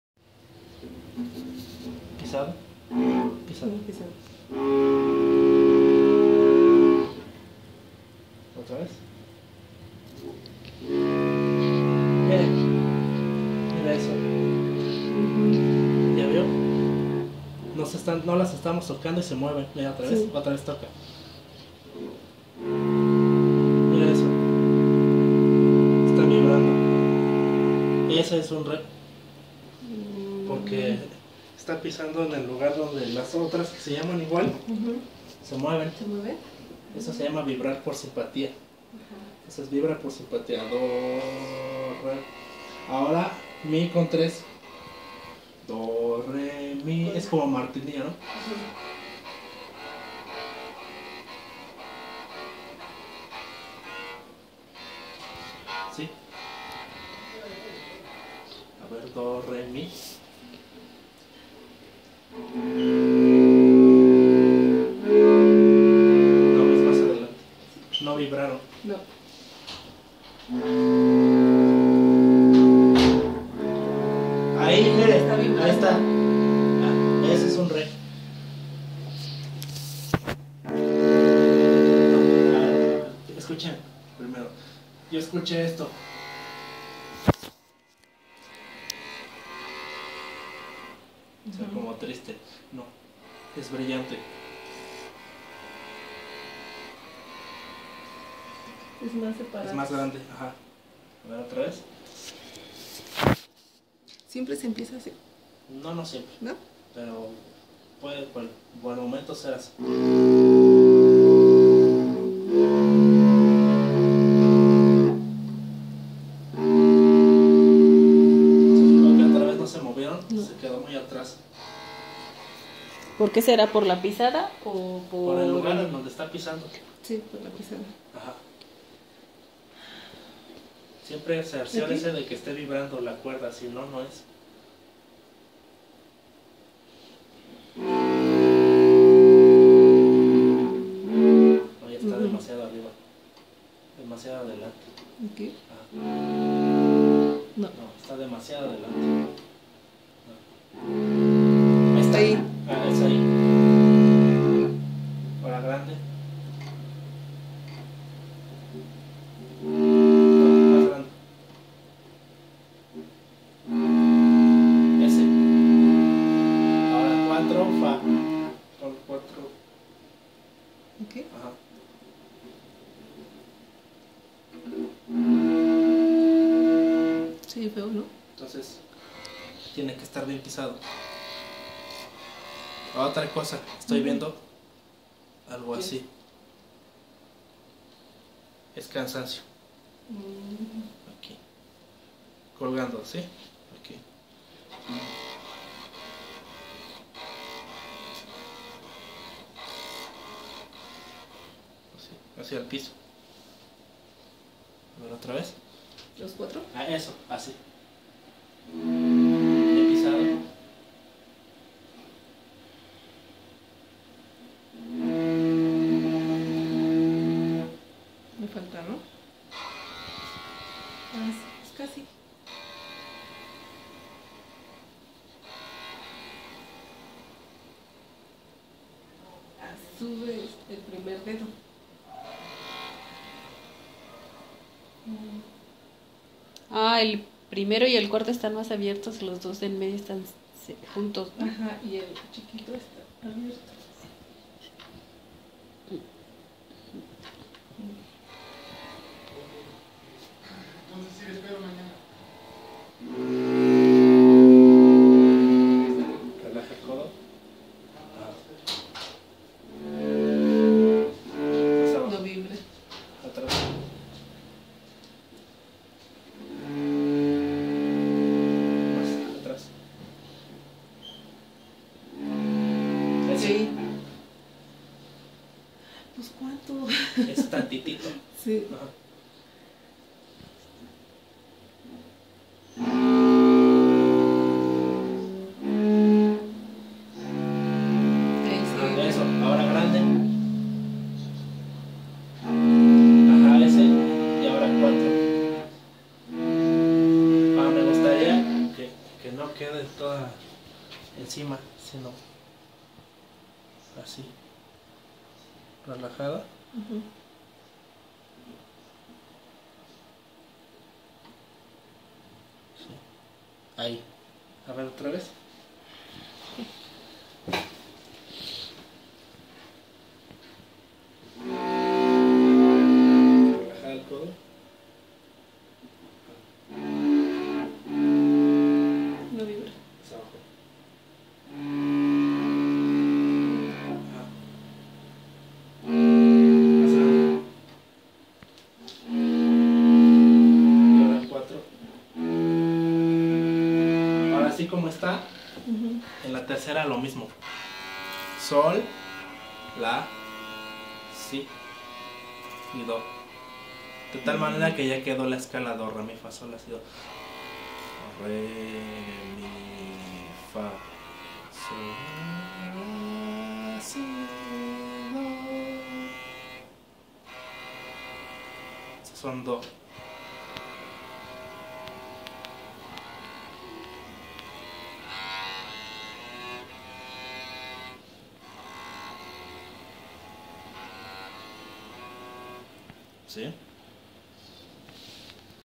Cómo se toca el Violoncello?
Caso: Inicia sin conocimientos, mayor de 40 años (parte 1)